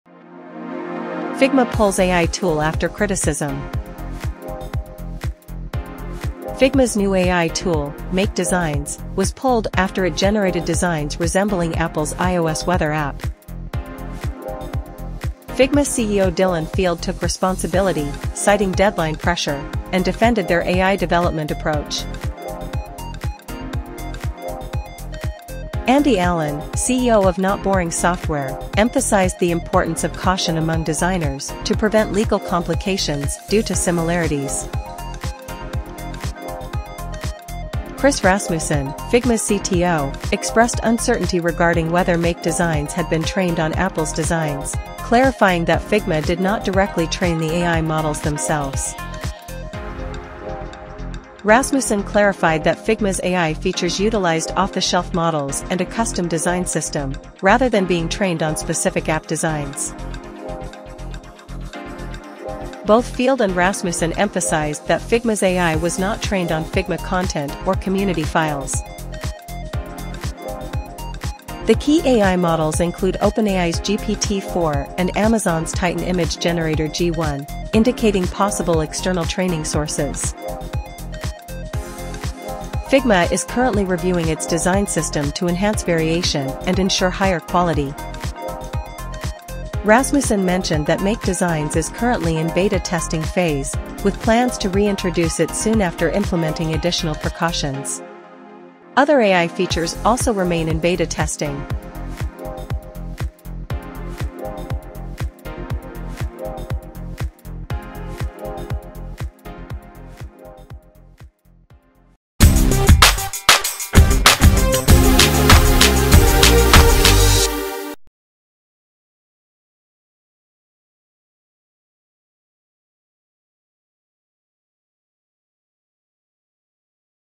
Figma Pulls Off AI Tool 'Make Designs'
Figma's latest AI tool, Make Designs, has been temporarily pulled after it generated designs strikingly similar to Apple's iOS weather app. 🚫📱 Dive into the controversy as Figma's CEO and CTO address the criticisms, explain the AI's training process, and share what's next for the popular design platform. Watch now to get the full scoop on this design debacle! 👀✨